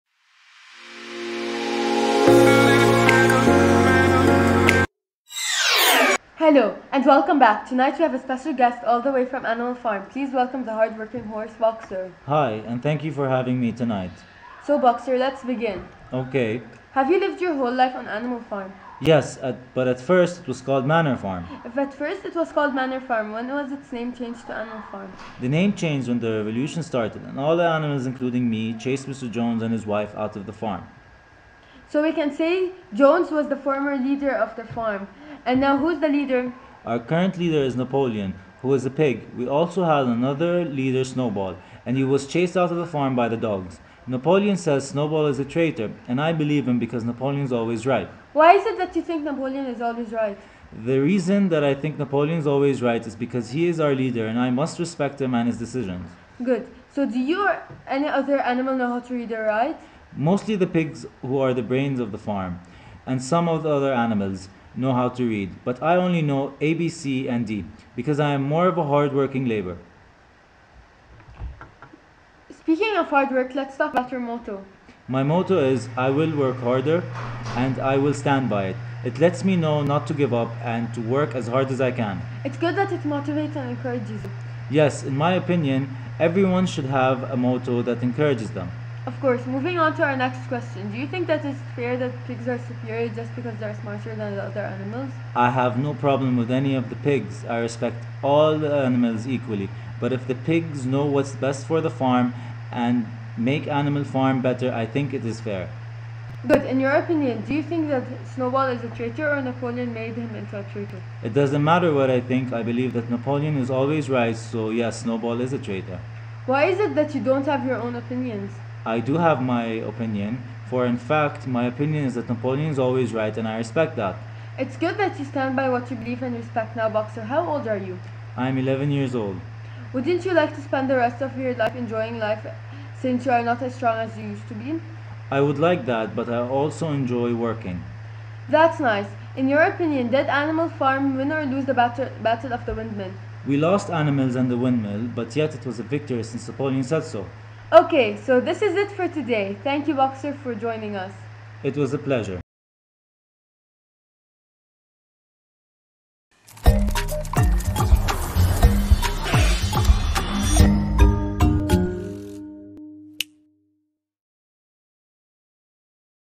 Kayla R's Animal Farm Interview Film
Watch a very creative interview conducted on the book Animal Farm by Kayla R.